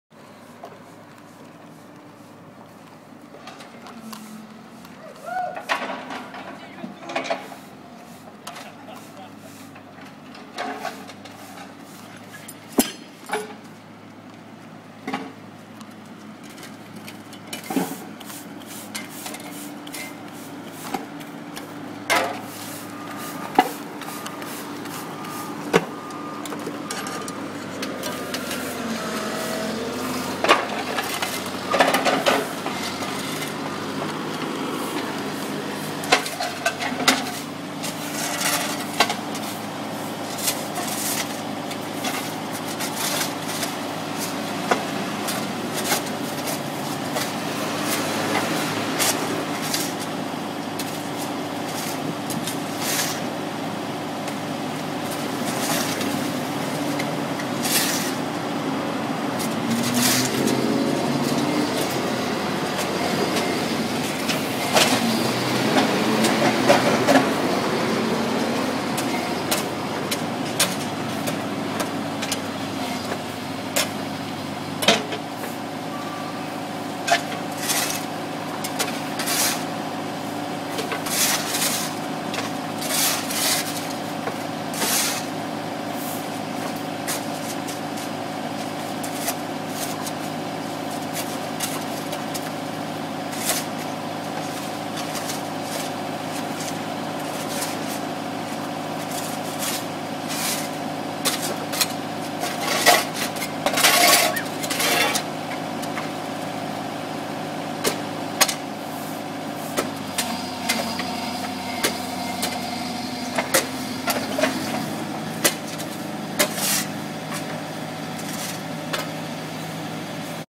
Mini Excavator road repair: Hollis St
Mini Excavator and crew repairing Hollis St, Emeryville CA on April 5, 2012